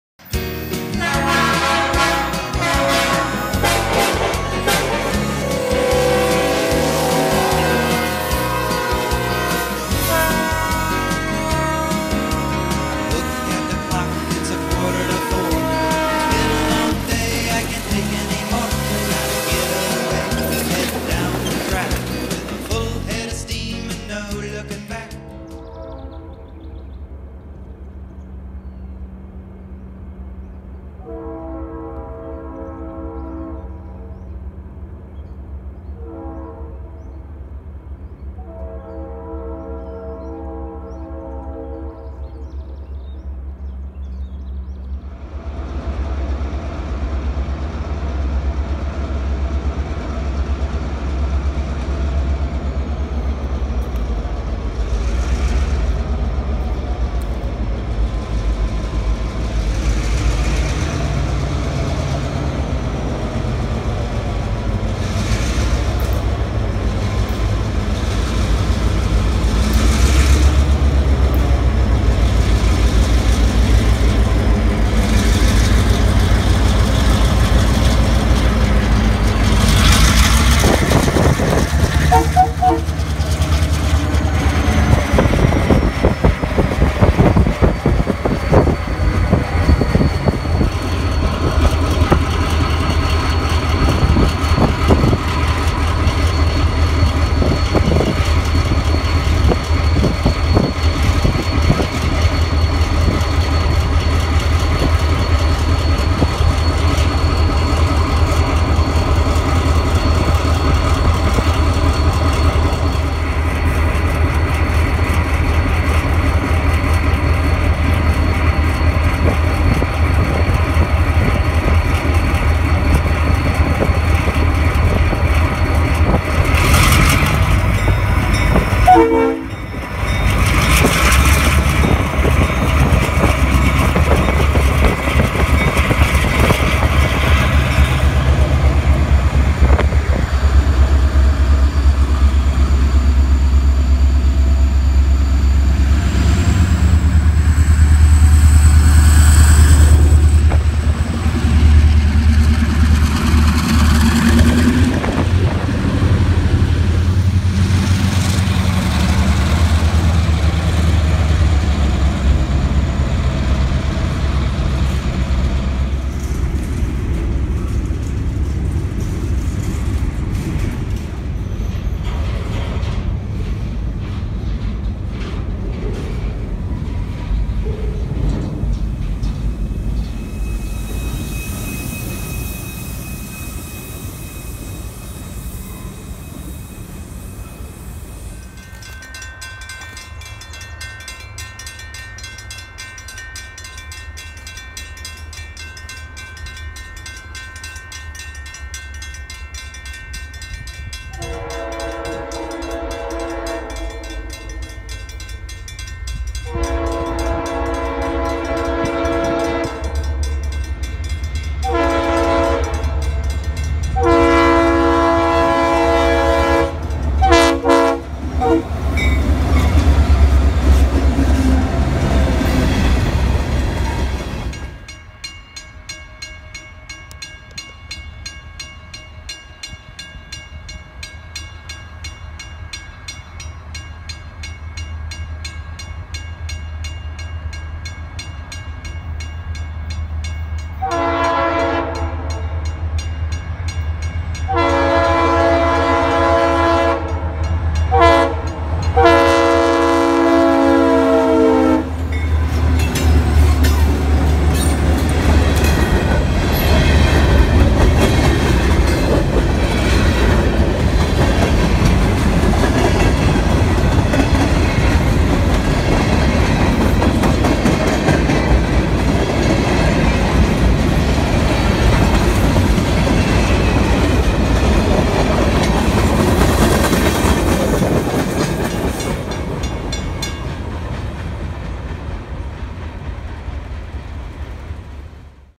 Veteran GP38-2 Chase - Close Overhead Views, Long Hood Running & More!
Here's an EMD GP38-2 that caught my eye right away! The old paint, the original NS logo, black painted number boards, (Now on most units, white, like the two trailing units) and a fantastic Leslie RS3L! Nothing could keep me away from giving chase to the great catch! As I stood atop an abandoned concrete bridge, the train makes a stop to let the conductor board - right under my position! I watched as the trio fired up, throwing hot air and smoke into my face as they pulled out of Portland. I then gave chase to two more locations with the engineer giving my friendly blasts along the way.